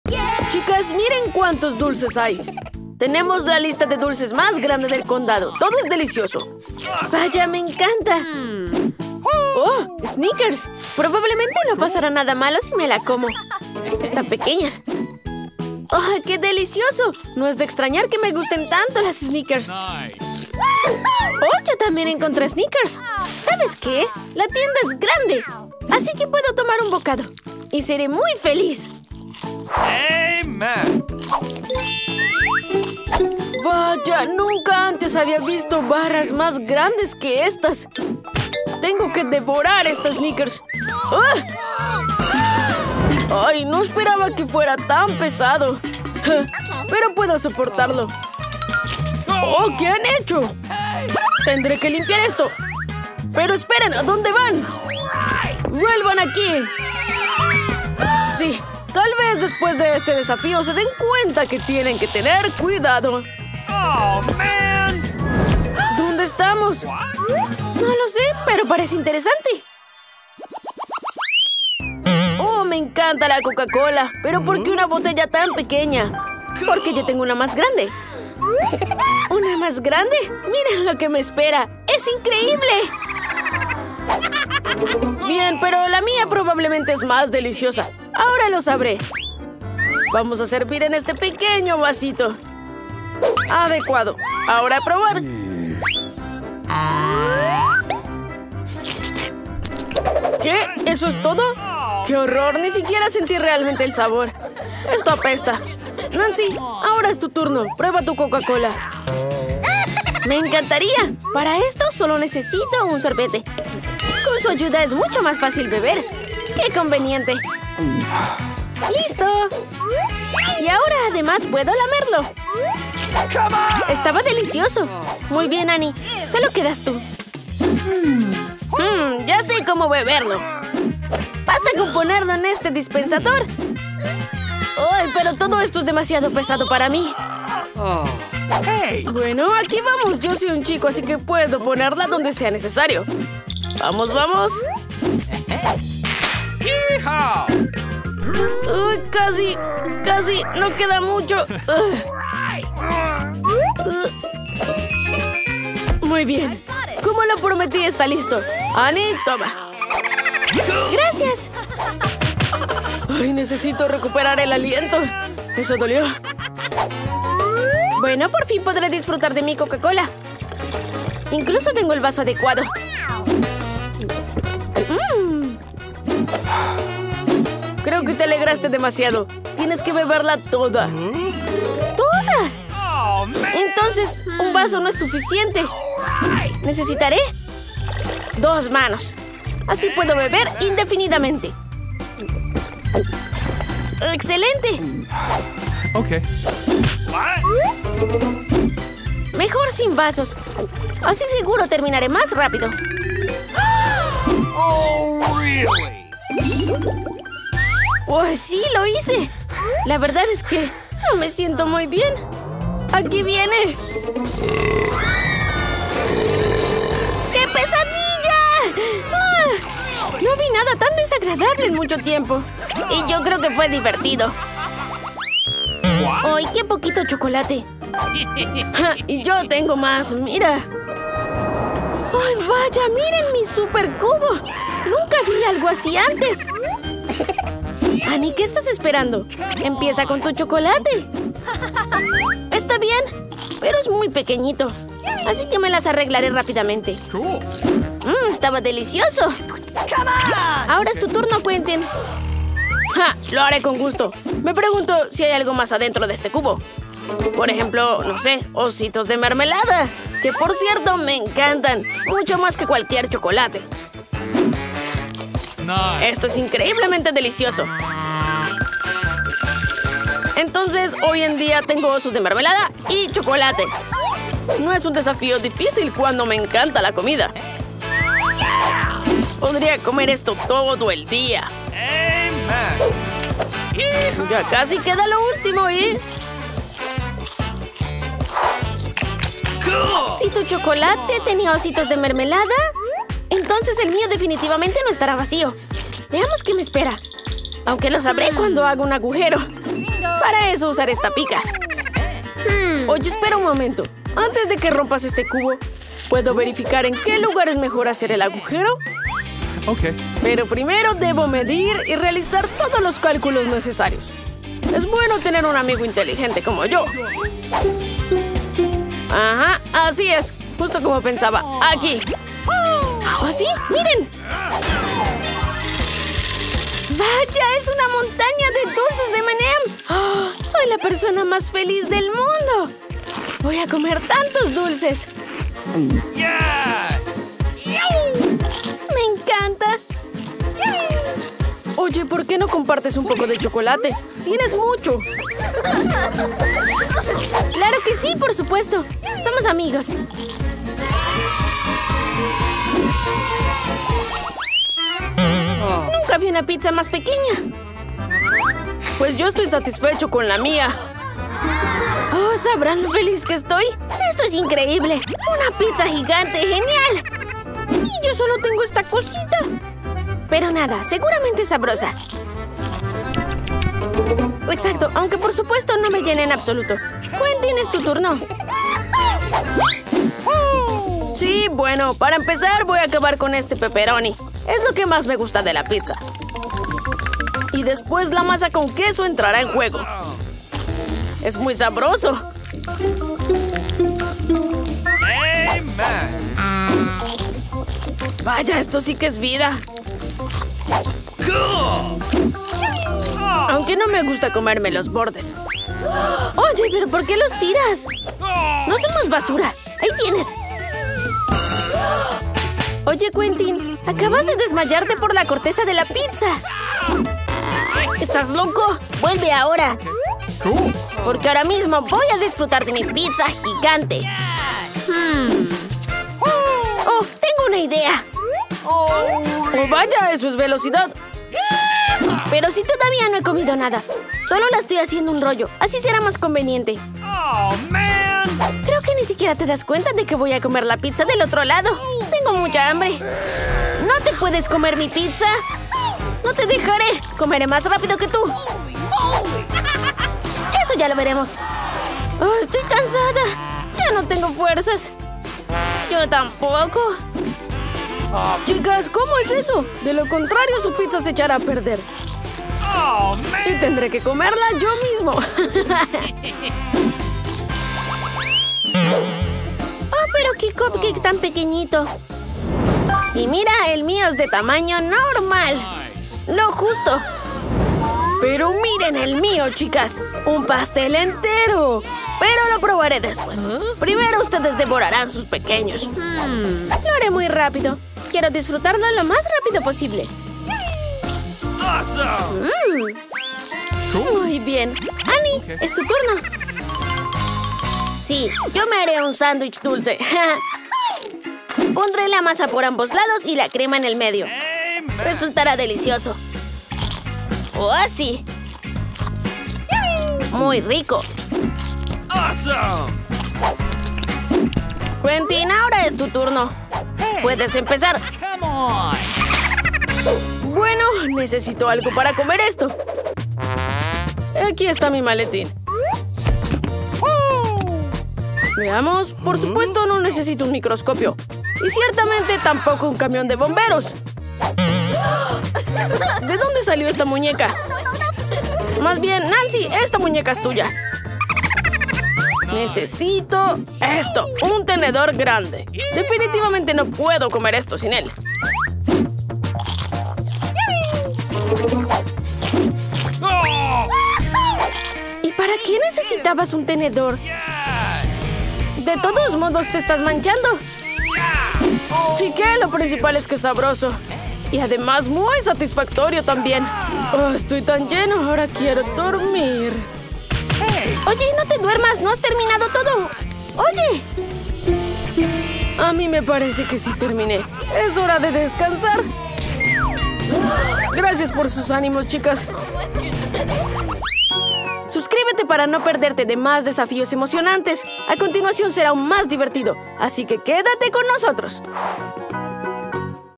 Desafío Placa Grande, Mediana y Pequeña | Desafíos de Comida Divertida PaRaRa Challenge
¿Quién crees que disfrutará la mayor porción de comida hoy? ¡Únete al nuevo y divertido desafío!